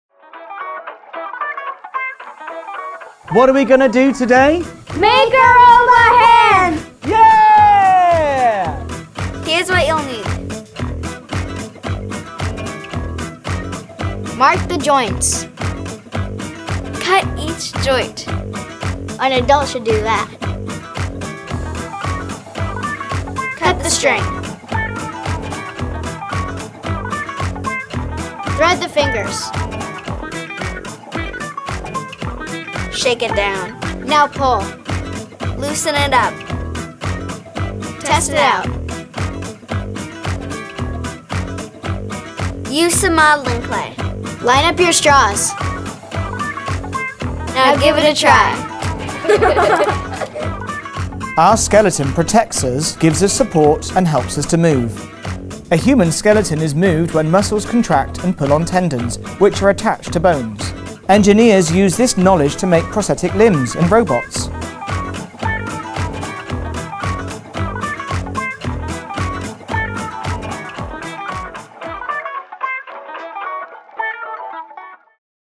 STEAM Project Ideas: How to Make a Robotic Hand
BISC Lincoln Park has some STEAM project ideas for you including how to make a robotic hand!

Watch the video then go behind the scenes to meet Raffi and Jacqueline as well as download step-by-step instructions for this science-based DIY (do-it-yourself) activity.